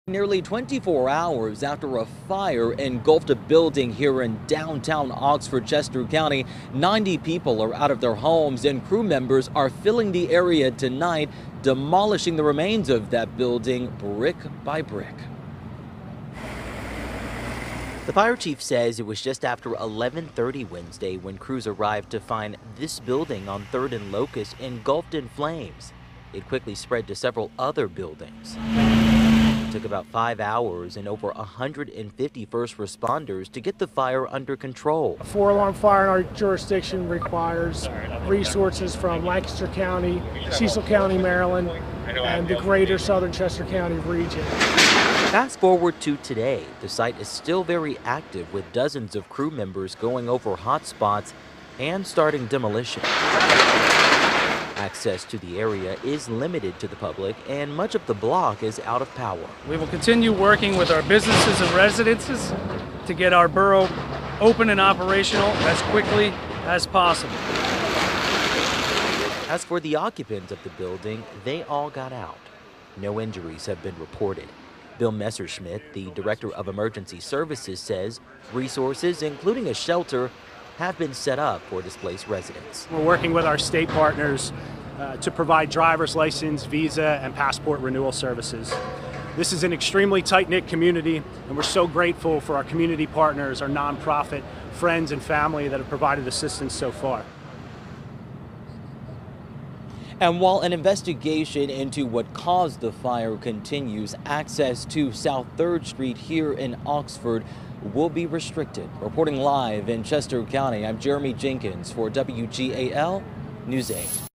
Fire-damaged buildings demolished in Chester County
A fire damaged dozens of apartments in Chester County forced 90 people from their homes.